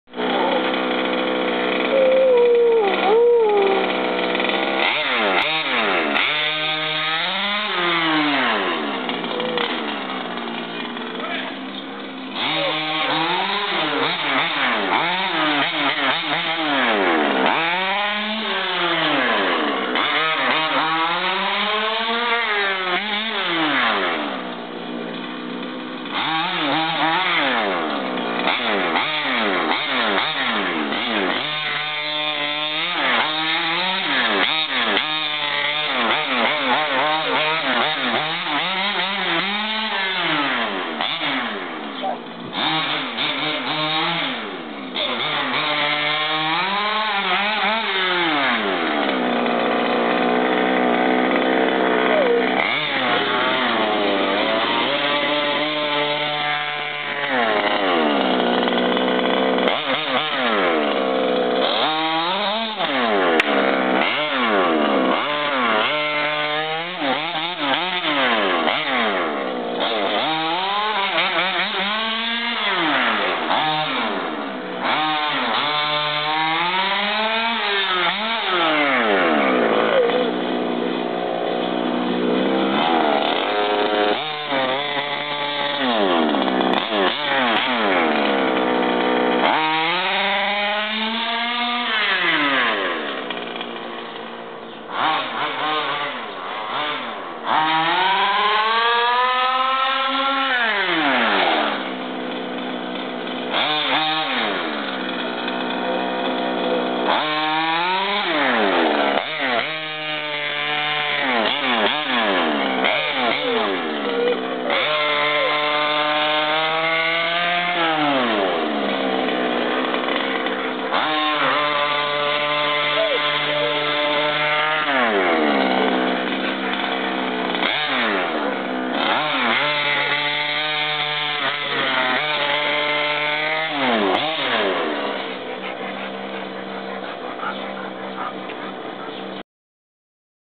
Baja 5B SS 3rd run On Road
May 31st, 1st day running 3rd run of the day.  This is a final evening run.  The forground sound effects is my dog wanting to play too.

Baja 5B SS with a 30.5cc stroker motor.  Pretty sweet!